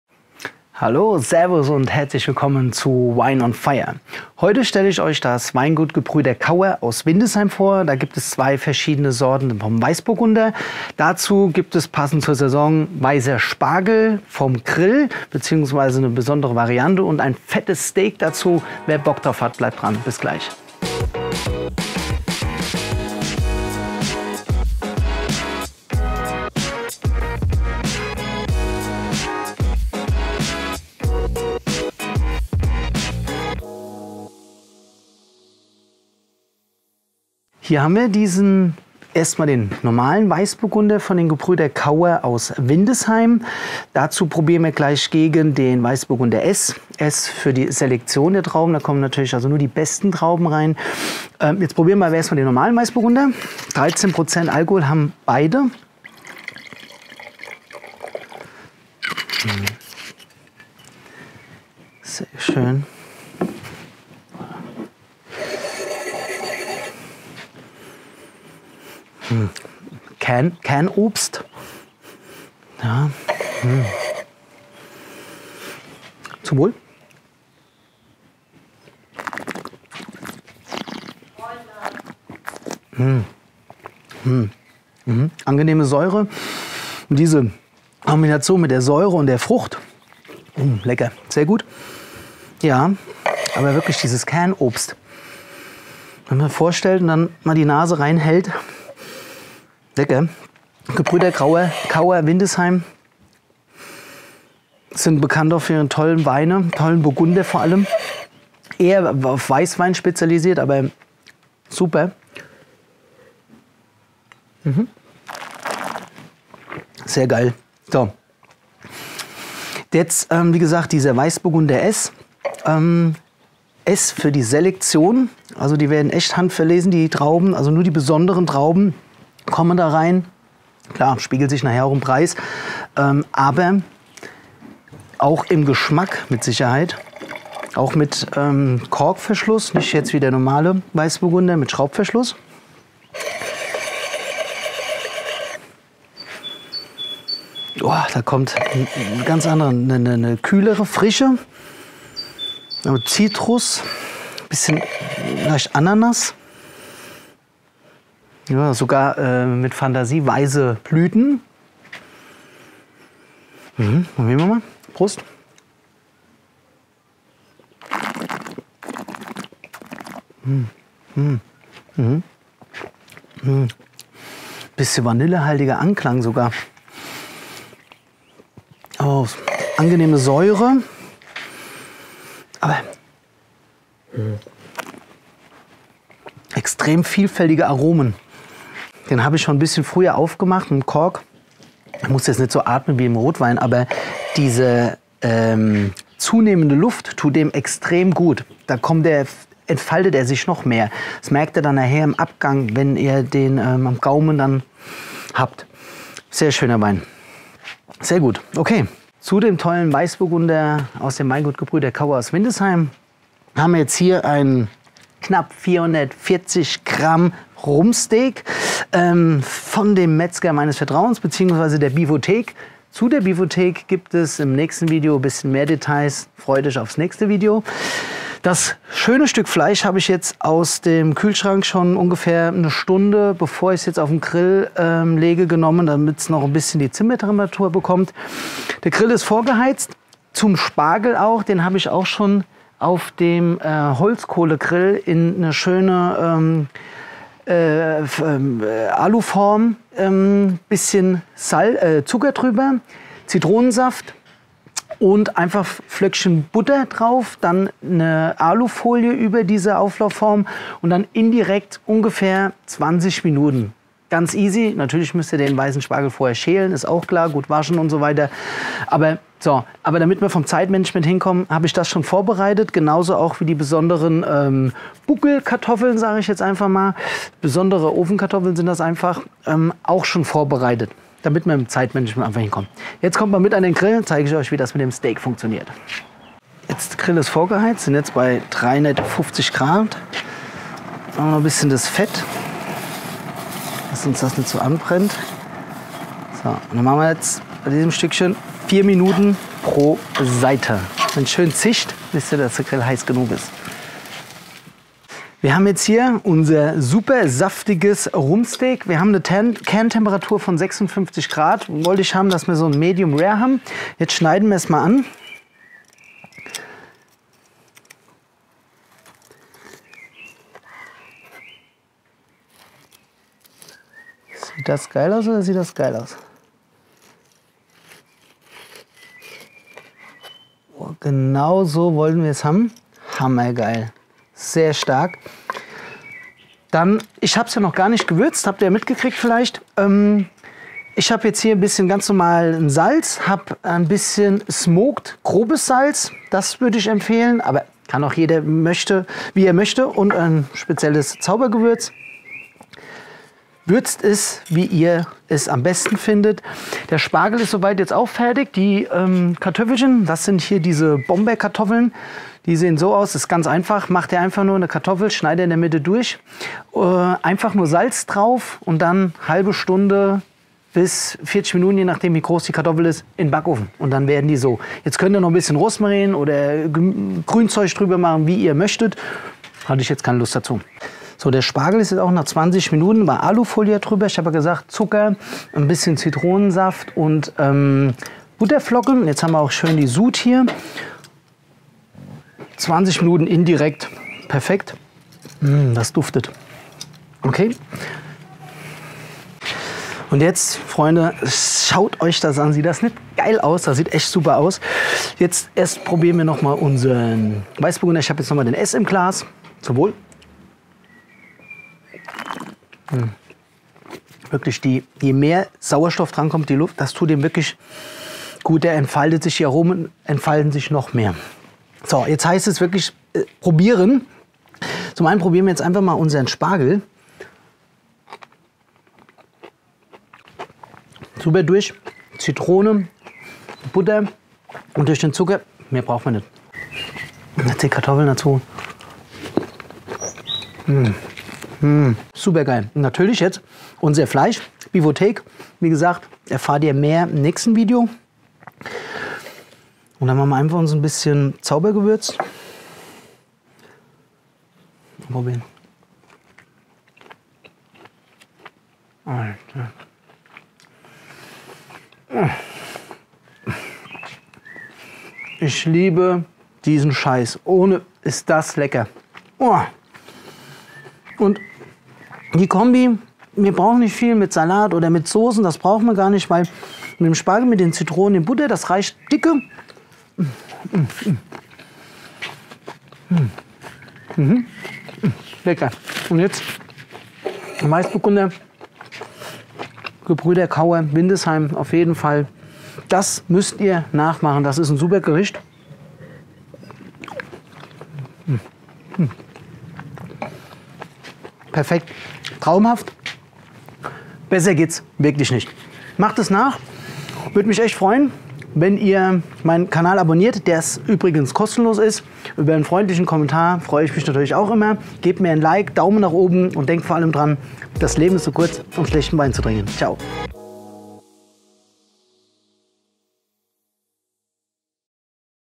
Rumpsteak, Spargel und Weißburgunder vom Weingut Gebr. Kauer
Heute stelle ich euch den Weißburgunder und den Weißburgunder-S- vom Weingut Gebrüder Kauer vor. Dazu gibt es passend zur Jahreszeit weißen Spargel und von der Beefothek ein knapp 440g Rumsteak.
Der Weißburgunder fasziniert mit seiner leichten Säure, aromatischen frischen und eleganten Art.  Der Weißburgunder-S- (SELECTION )– mit seinem herrlicher Duft nach weißen Blüten, gepaart mit feiner Frucht von Melonen und einem mundausfüllenden Schmelz. Ein echt toller Wein!!!

Die Familie Kauer widmet sich schon seit mehreren Jahrhunderten dem Thema Wein. Das Weingut in Windesheim ist seit dem Jahr 1697 in Familienbesitz. Mit dem Einstieg von Markus Kauer im Jahr 1992 übernimmt die 14te Generation das Ruder. Seit dem Jahr 2000 steht ihm sein Cousin Christoph zur Seite, der im gleichen Jahr Leitung der Kellerwirtschaft übernimmt.
Das sagen die Winzer: "Durch naturnahen Anbau geben wir der Rebe die Möglichkeit, natürlich zu wachsen, um Trauben höchster Güte hervorzubringen." Die Kauers sind Winzer mit hohen Ansprüchen und der Vision die Qualität der angebotenen Weine immer weiter zu verbessern.
Das Weingut wird in den renommierten Weinführern Guide Gault Millau, Eichelmann und Vinum geführt.
Wer mehr über dieses Weingut bzw. den Wein probieren möchte, kann sich hier auf der Homepage informieren:

Zutaten für 1-2 Personen:
1x Rumsteak mind. 300g
1 Bund weißer Spargel
2 mittelgroße Kartoffeln
Butter (3 Flocken, ca. 50g)
1 Zitrone
Zucker
Salz
Pfeffer

Zubereitung:
die Kartoffeln waschen und in der Mitte längs durchschneiden. Auf die jeweiligen Hälften gut Tafelsalz bestreuen und für ca. 30 bis 40 Minuten in den Backofen mit der Salzseite nach oben mit Ober/Unterhitze bei 200Grad backen.
Den Spargel schälen, in eine Auflaufform geben, dass der Boden bedeckt ist. Etwas ca. 1 EL Zucker über den Spargel geben, mit dem Saft einer halben Zitrone beträufeln und dann die Butter über den Spargel verteilen, ruhig grob, da die Butter auf dem Grill schmilzt. Die Auflaufform mit Alufolie zudecken und für ca. 20 Minuten mit indirekter Hitze auf den Grill geben.
Das Steak ca. 1 Stunde bevor es auf den Grill kommt aus dem Kühlschrank auf einen Teller mit Zewa abgedeckt ruhen lassen.
Den Grill gut vorheizen und das Steak ca. 4 Minuten pro Seite Angrillen, danach das Steak ca. 5 bis 8 Minuten weitergaren lassen ohne Flamme, bis wir eine Kerntemperatur von ca. 56 Grad (Medium) erreicht haben. Vom Grill runter und nochmal für 2 bis 3 Minuten ruhen lassen. Danach gehts ans Anrichten.
Guten Hunger!!

Euer Lars